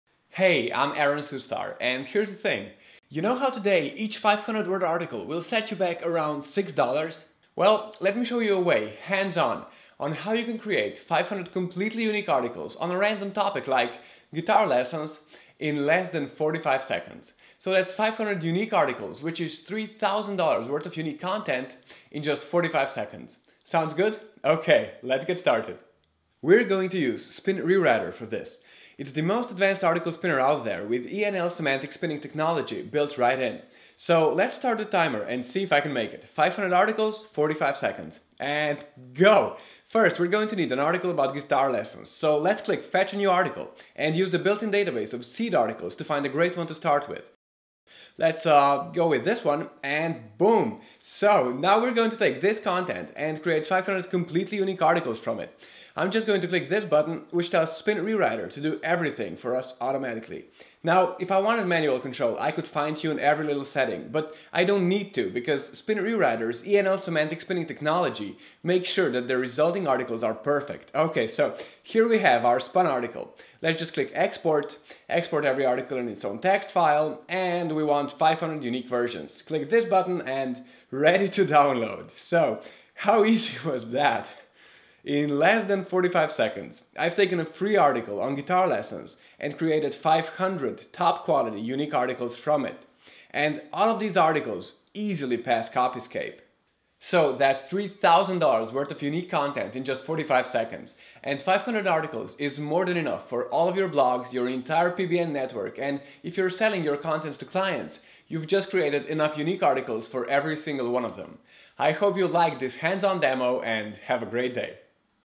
Time Challenge: 500 Articles in 45 Seconds
Hey, it's Aaron Sustar -- and here's the thing.

You know how today each 500-word article will set you back about 6 dollars?

Well, let me show you a way – hands on! – how to create 500 completely unique articles on a random topic (like "guitar lessons") in less than 45 seconds.

So that’s 500 unique articles, or 3000 dollars worth of unique content  -- in just 45 seconds.

We’re going to use Spin Rewriter for this. It's the most advanced article spinning software out there, with the ENL Semantic Spinning technology built right in.

So, let’s start the timer and see if I can make it.

500 articles, 45 seconds. GO!

First, we’re going to need an article about guitar lessons. Let’s click “Fetch a New Article” and use the built-in database of seed articles to find a great one to start with.

Now we’re going to take this content and create 500 completely unique articles from it. I’m just going to click this button which tells Spin Rewriter to do everything for us automatically.

Now, if I wanted manual control, I could fine-tune every little setting… but I don’t need to, because Spin Rewriter’s ENL Semantic Spinning technology makes sure that the resulting articles are perfect.

Done. Here we have our spun article… Let’s just click “Export”…

Export every article in its own text file… We want 500 unique versions.

Click the button and... “Ready to download...”

How easy was that? :)

In less than 45 seconds, I’ve taken a free article on "guitar lessons"  and created 500 top quality unique articles from it.

And all of these articles easily pass Copyscape.

That’s 3000 dollars worth of unique content in 45 seconds…

And 500 articles is enough for all your blogs, your entire PBN network, and if you’re selling your content to clients, you’ve just created enough unique articles for all of them.

I hope you liked this quick hands-on demo, and – have a great day!